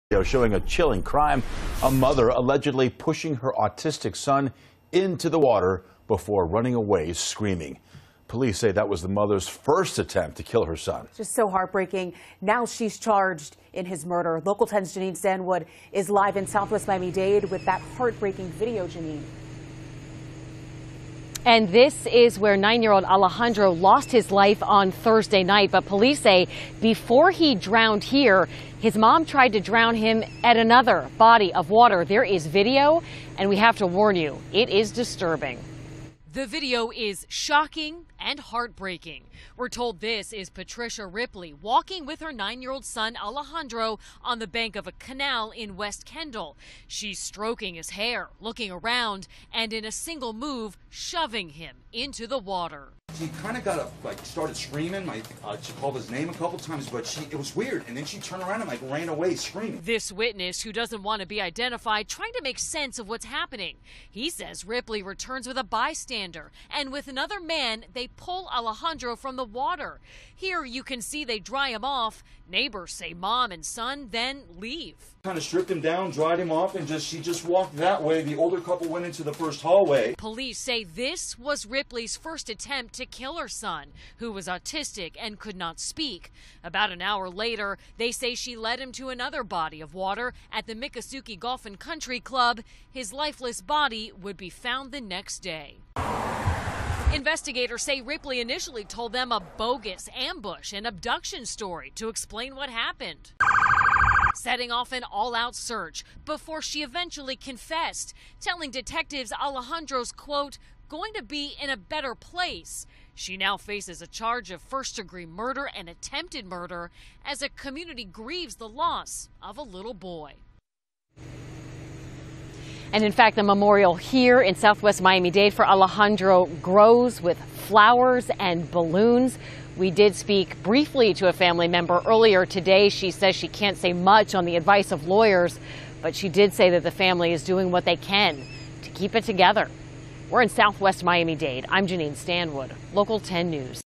Horrible: Woman Charged With Murder After Throwing Her Autistic Son In A Lake
Days after Miami’s Patricia Ripley was charged with her autistic son’s murder, shocking surveillance video of her apparently pushing the child into a canal has emerged. The video, obtained and aired by Univision’s national news network, shows Ripley walking 9-year-old Alejandro on the bank of a West Kendall canal, looking around, stroking his head — and then forcefully shoving him into the water. Then, Ripley runs away off screen. About 20 seconds later, she returns accompanied by a bystander who saw the child in the murky water and rushed to rescue him. Authorities say the shove was Ripley’s first, but thwarted, attempt to kill Alejandro Thursday evening. About an hour later, with no bystanders to rescue him, the boy was led into another canal, where he ultimately died, Miami-Dade State Attorney Katherine Fernandez Rundle has told reporters. The death of Alejandro, who suffered from severe autism and could not speak, has enraged the public — along with Ripley’s bogus report to Miami-Dade police that two black men kidnapped the boy after running her off the road that night.